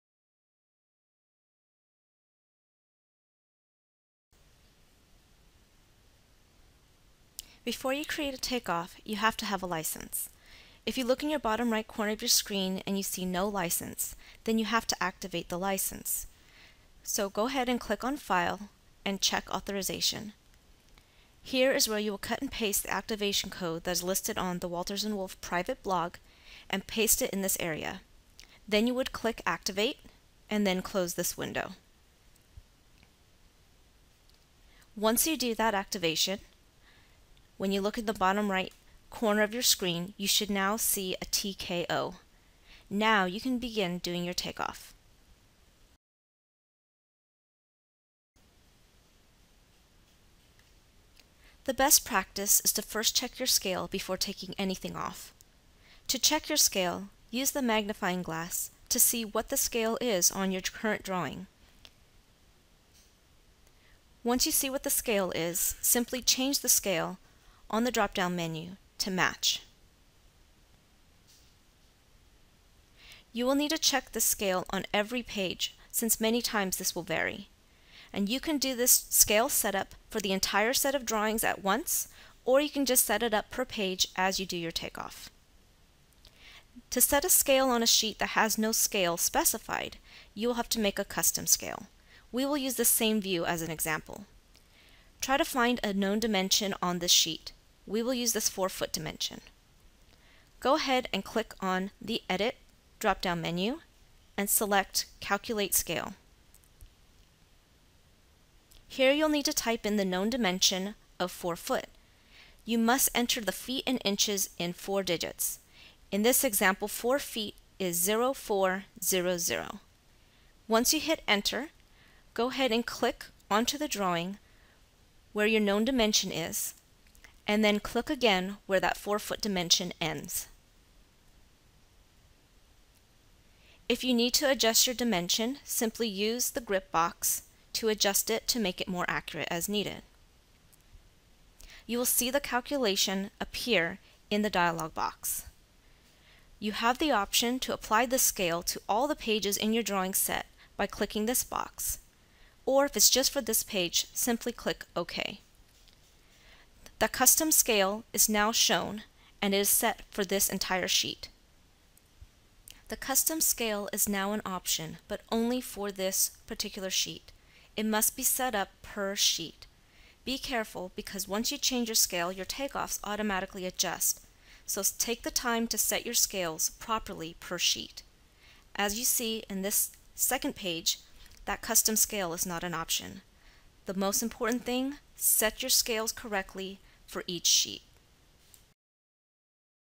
OST tutorial | (1) Activation and Set the Scale
On Screen Takeoff W&W tutorial:  First activation your license.  Then set your scales - which is one of the most important things to do.